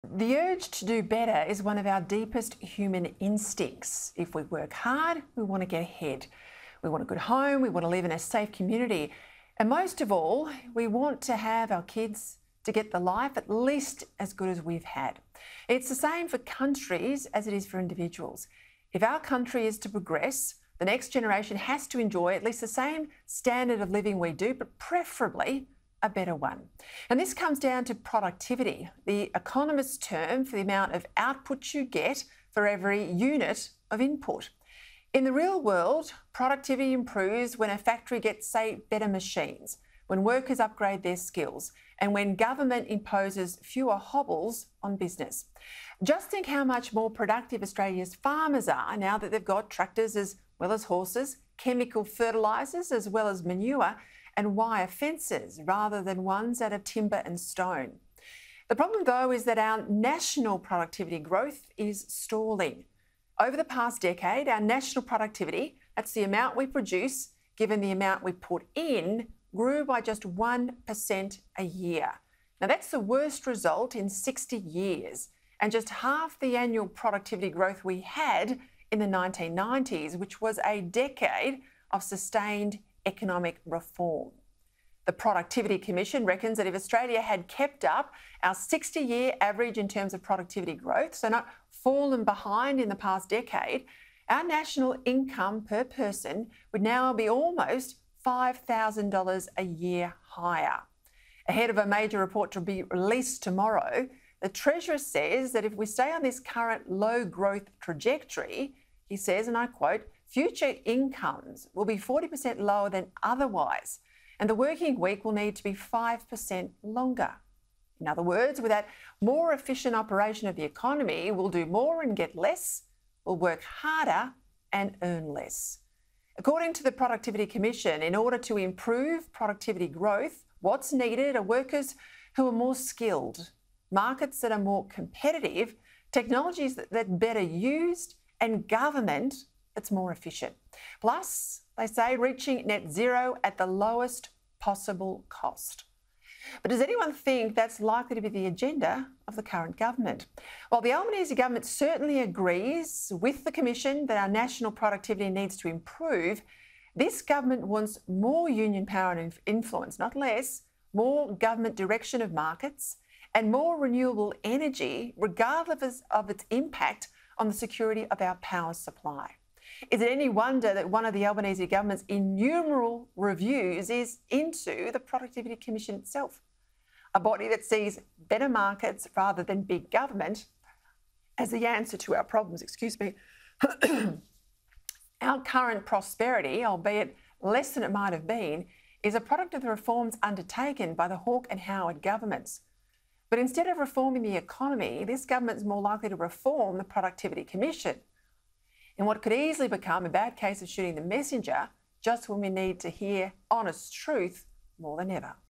Govt 'more likely' to 'reform' Productivity Commission than the economy: Credlin
Sky News host Peta Credlin says instead of reforming the economy, the government is more likely to reform the Productivity Commission following the news our national productivity growth is stalling.

"Over the past decade, our national productivity – that's the amount we produce, given the amount we put in – grew by just one per cent a year," Ms Credlin said.

"That's the worst result in 60 years and just half the annual productivity growth we had in the 1990s, which was a decade of sustained economic reform.

"While the Albanese government certainly agrees with the commission that our national productivity needs to improve, this government wants more union power and influence … more government direction of markets, and more renewable energy, regardless of its impact on the security of the power supply.

"Is it any wonder that one of the Albanese government's innumerable reviews is into the Productivity Commission itself – a body that sees better markets, rather than big government, as the answer to our problems?"